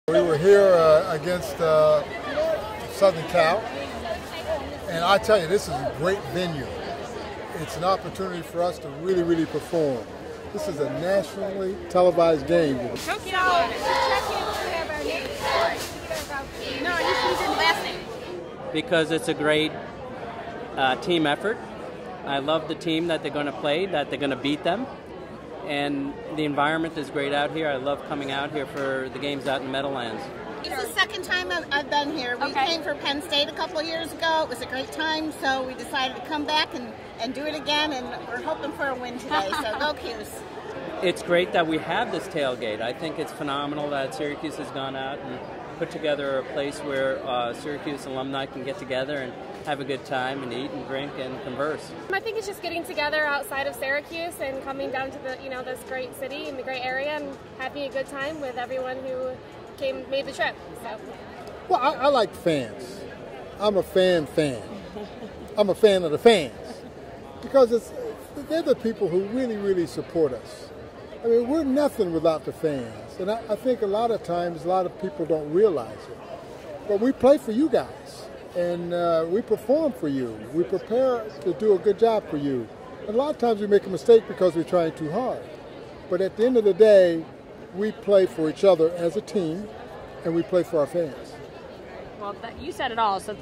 Orange Club Hosts Ultimate Tailgate at MetLife Stadium - Syracuse Football
Sights and sounds from the the New York's College Team Ultimate Tailgate at MetLife Stadium, hosted by the Orange Club before Syracuse football's game vs. Notre Dame on September 27th, 2014.